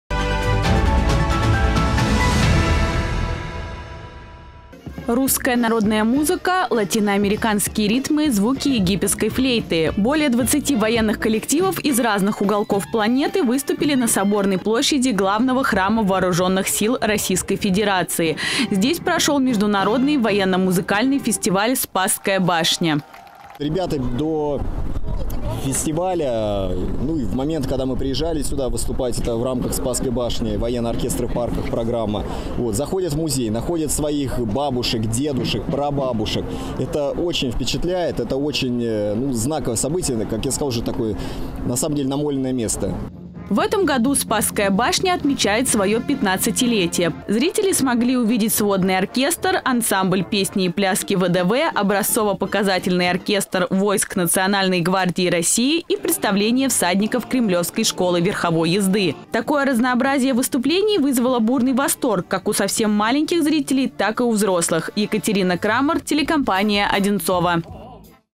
Фестиваль «Спасская башня»
На Соборной площади главного храма вооружённых сил РФ прошла дневная программа Международного военно-музыкального фестиваля «Спасская башня». Здесь выступили более 20 военных коллективов и порадовали зрителей своими исполнениями.